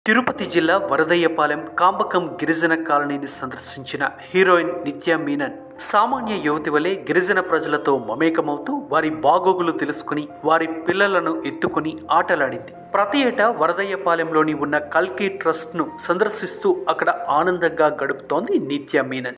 Heroine Nitya Menon Playing with small children at Varadaiahpalem
Watch :: Heroine Nitya Menon Playing with small children at Varadaiahpalem | @viswabharathtv